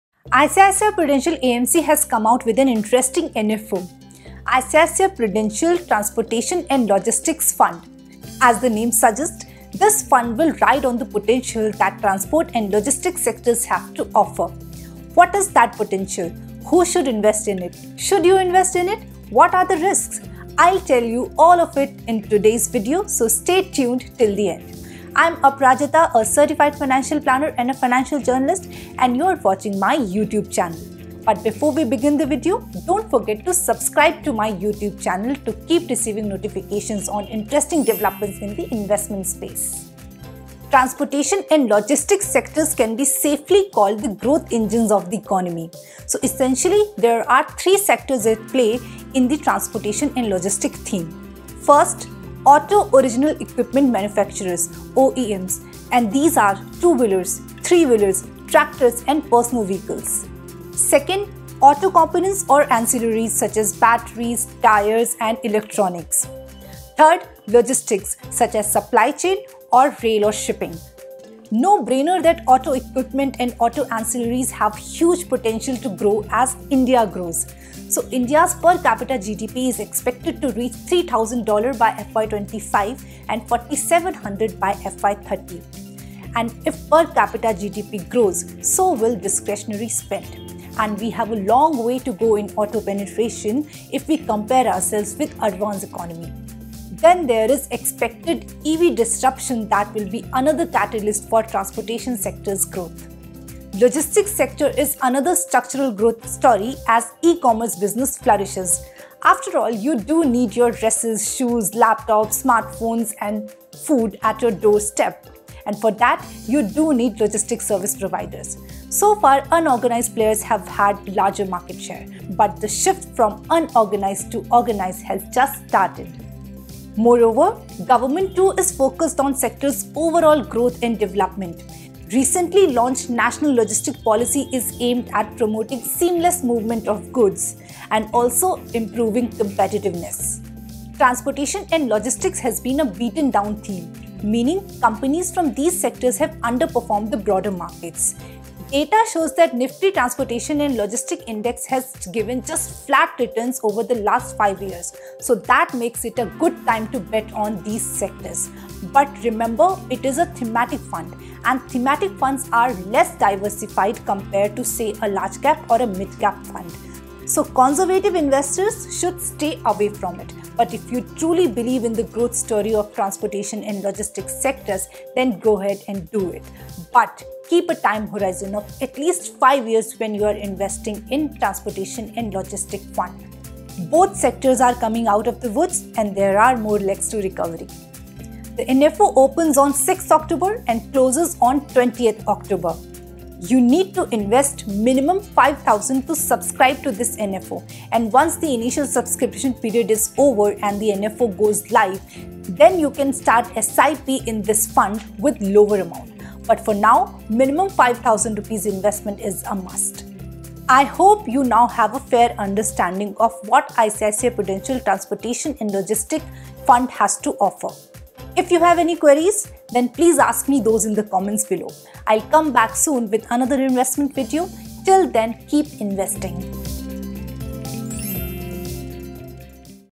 ICICI Prudential Transportation & Logistics NFO | Know details
ICICI Prudential Transportation & Logistics NFO: Transportation and logistics can be safely called the growth engine of the economy. Essentially, there are three sectors at play in the said NFO –

i) Auto Original Equipment Manufacturers (OEMs) such as 2W, 3W, Tractors, PVs etc.
ii) Auto Components or Ancillaries) such as Batteries, Electronics and Tyres, etc
iii) Logistics such as Supply chain and Rail, Shipping, etc.

No brainer that auto equipment and auto ancillaries have a huge potential as India grows. Country’s per capital GDP is expected to reach $3000 mark by FY25E and $4700 by FY30E. As per capita GDP grows so will the discretionary spend. We have long way to go in terms of auto penetration if we compare ourselves with advanced economies. Then there is expected EV disruption that will be another catalyst to the transportation sector’s growth.

Logistics sector is another structural growth story as the e-commerce business flourishes. Afterall, you need logistic service providers to get your dresses, shoes, phones, laptops and food delivered at your door step. So far unorganised players have had the largest market share, but the shift from unorganised to organised sector has started. Moreover, government too is focussed on the sector’s overall growth & development. Recently launched National Logistics Policy (NLP) is aimed at promoting seamless movement of goods & improving competitiveness.

Should you invest?

Transportation and logistics has been a beaten down theme. Meaning companies from these sectors have underperformed the broader markets. Data shows that Nifty Transportation & Logistics Index has delivered almost flat returns in last 5 years.

That makes it a good time to bet on these sectors. But remember, it is a thematic mutual fund. Such mutual funds are exposed to only one theme so these are less diversified compared to a largecap or a midcap fund. So keep this in mind. Also make sure that you invest in it with a time horizon of at least five years. Conservative investors should stay away from it. But if you truly believe in the growth story of transportation and logistics, then go ahead and do it. Both sectors are coming out of the woods and there are more legs to recovery.

The NFO Details

The NFO opens on October 6th and closes on October 20th. Once the NFO goes live after the subscription, you may start SIP in the same with the lower amount. But for now, minimum Rs 5,000 is a must.